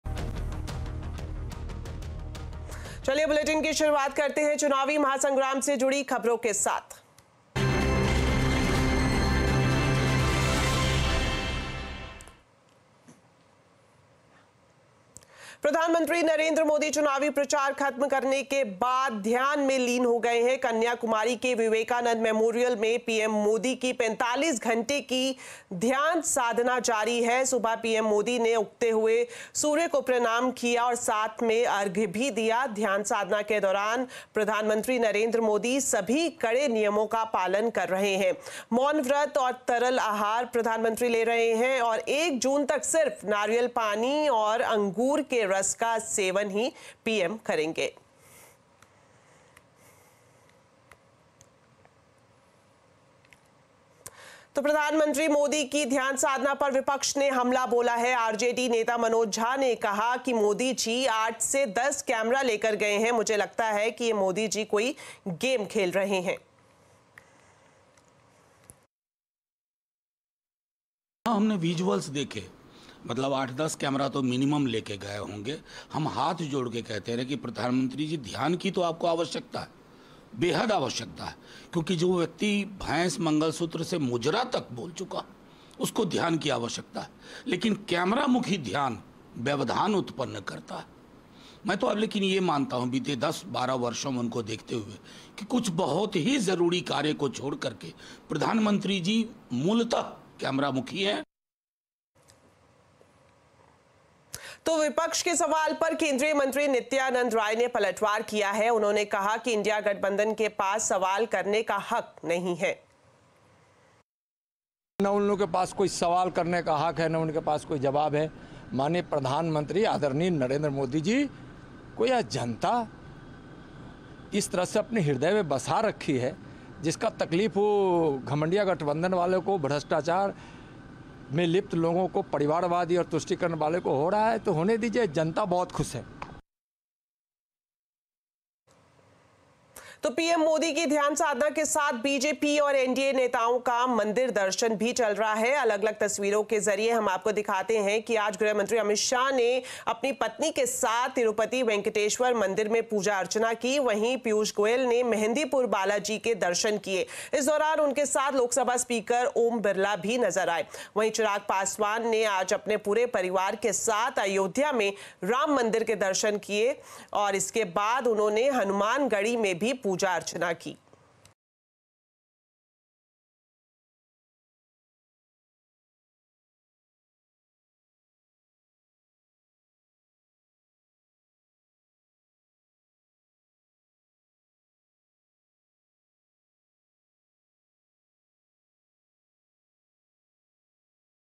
PM Modi Meditation | Kanyakumari में PM मोदी का ध्यान साधना | Lok Sabha Election | BJP
PM Modi Meditation: प्रधानमंत्री नरेंद्र मोदी का ध्यान आज चर्चा के केंद्र में है. उन्होंने शुक्रवार को सुबह मां अम्मन मंदिर में पूजा अर्चना की और इसके बाद विवेकानंद रॉक मेमोरियल के ध्यान केंद्र में ध्यान साधाना में तल्लीन हो गए. मीडिया रिपोर्ट के मुताबिक प्रधानमंत्री नरेंद्र मोदी 48 घंटे तक ध्यान शाधना में तल्लीन रहेंगे. ध्यान भारत की बहुत प्राचीन परंपरा है और योग का अगला पड़ाव.